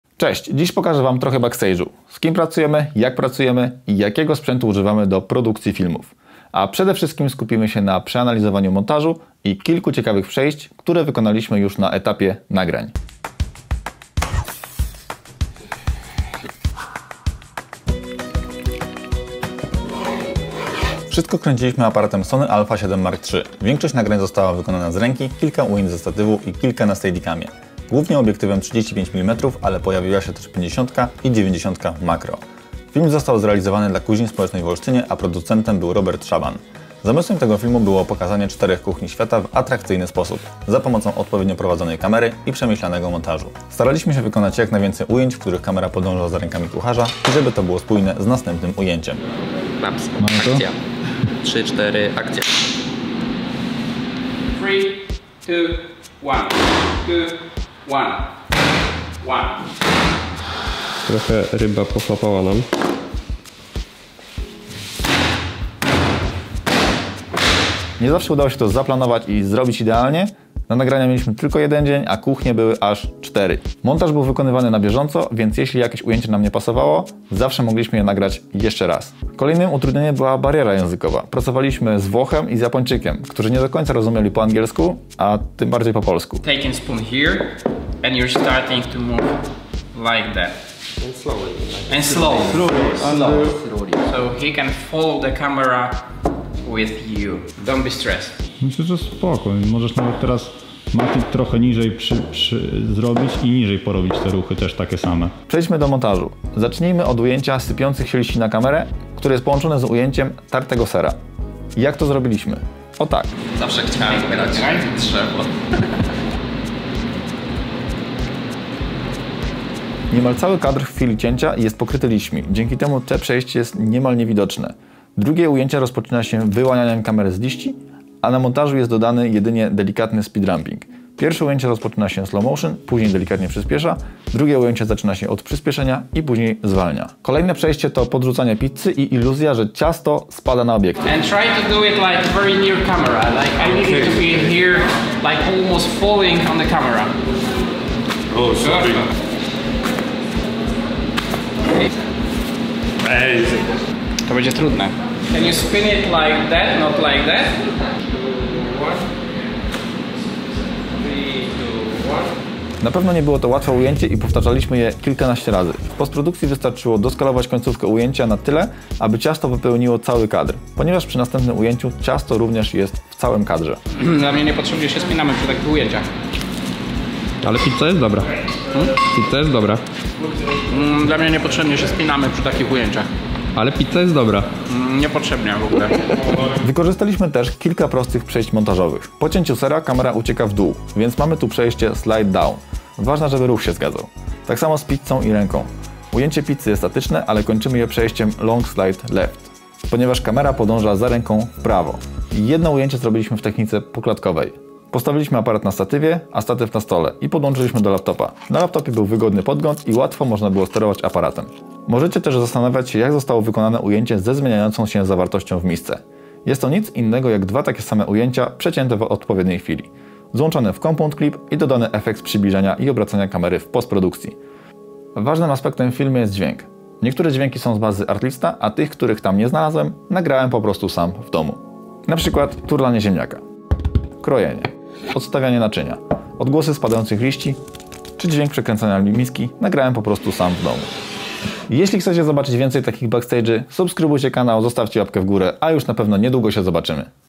Kulisy produkcji filmu - Kuchnia Warsztatowa
Dziś pokażemy Wam trochę zaplecza filmowego i opowiemy o technice montażu potocznie zwanej "B-Rollem" na podstawie produkcji filmu dla Kuźni Społecznej w Olsztynie. Jakim aparatem nagrywamy, jakich obiektywów używamy i jakie trudności napotkaliśmy podczas produkcji. To wszystko w dzisiejszym filmie 🎥